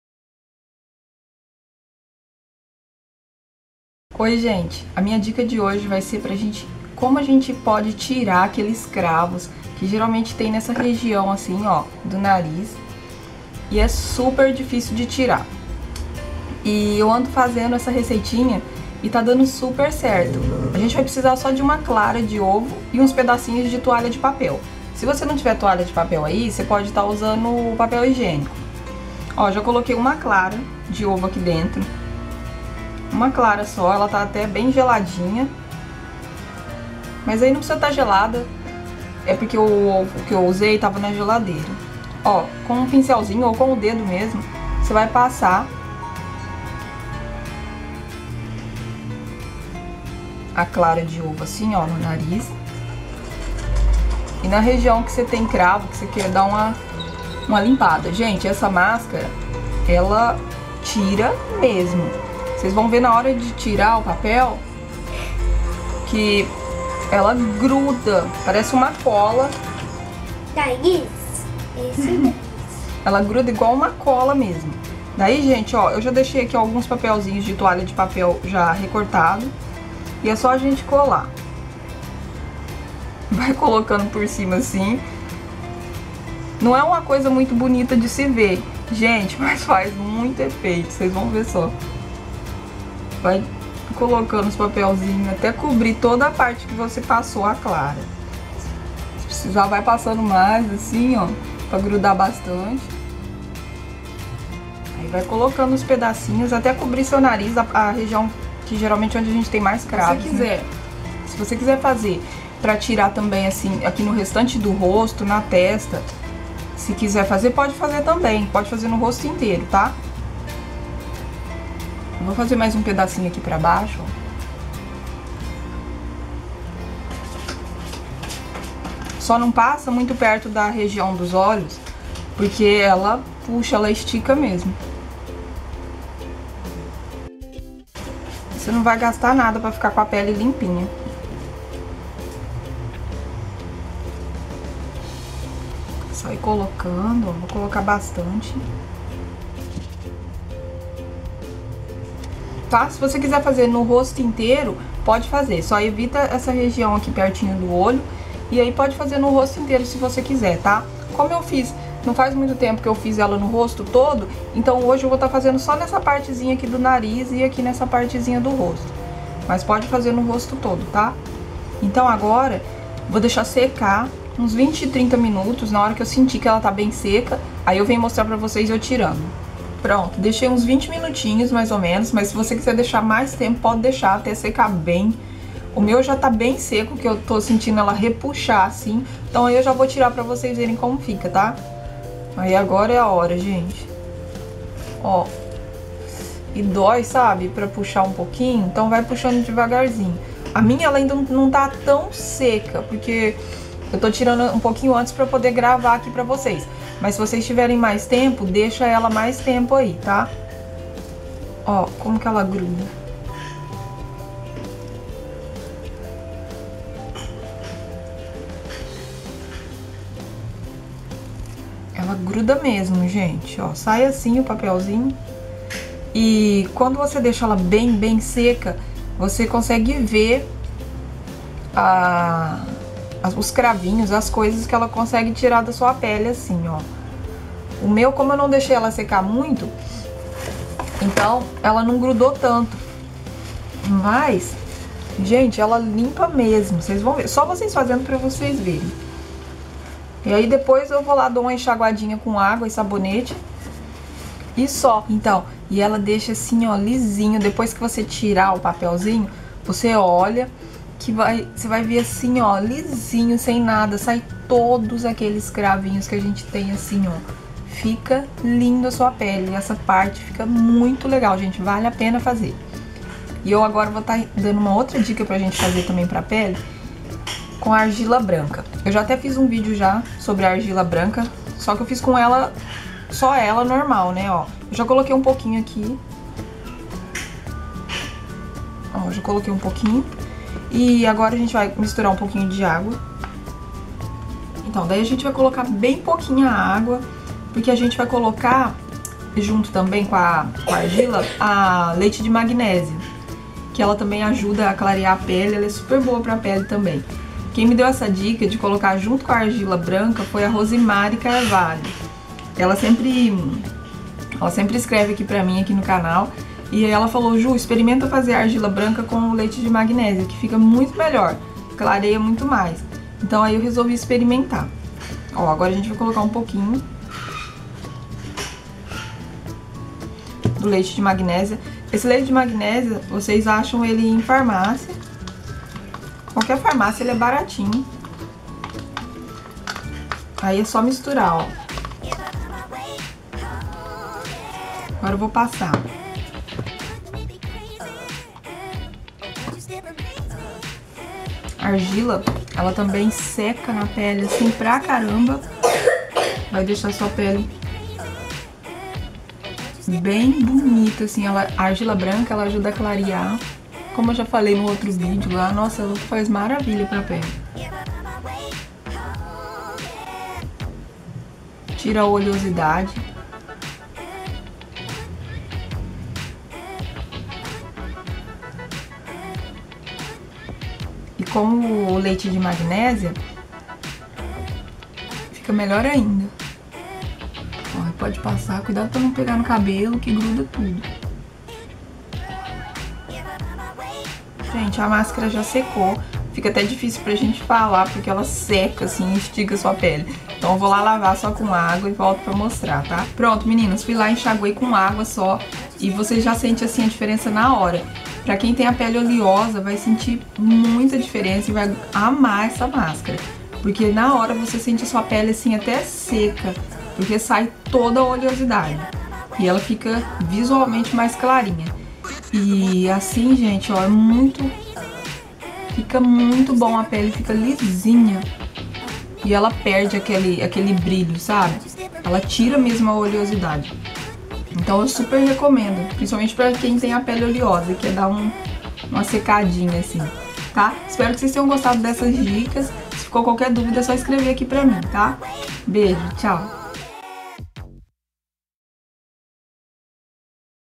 Como eliminar os cravos e clarear a pele(duas dicas caseira)
minha pagina com dicas de beleza no facebook

meu instagram:  @juliana.santos.simplicia